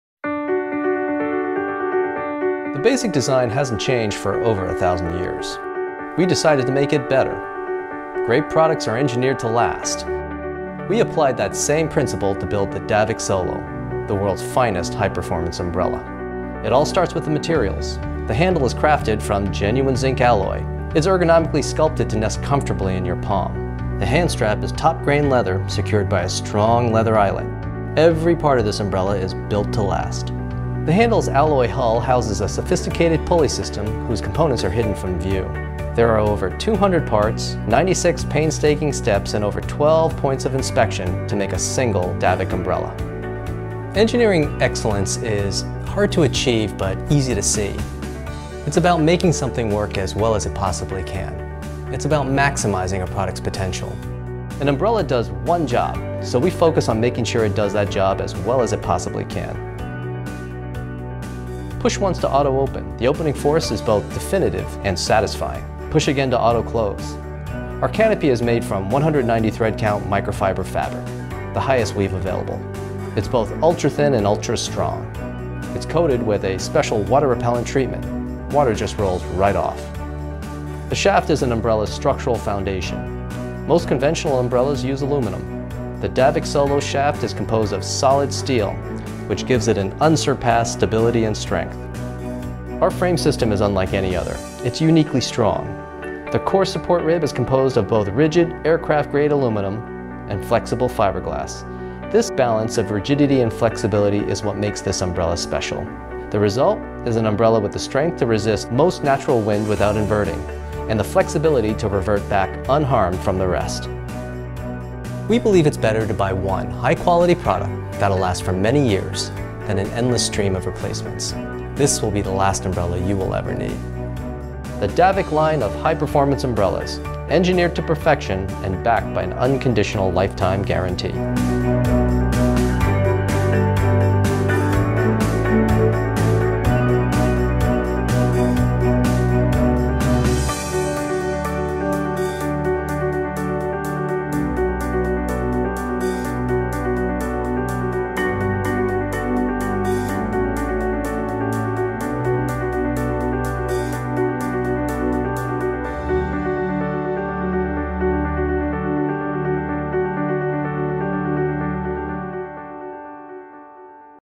Davek Solo - The World's Finest High-Performance Umbrella
Storyboard Media Group was approached by the Founder and CEO of Davek Accessories to make a marketing video about the Davek Solo - the client's flagship umbrella. This product video shows off each and every part of an umbrella designed with comfort, stability, and engineering in mind.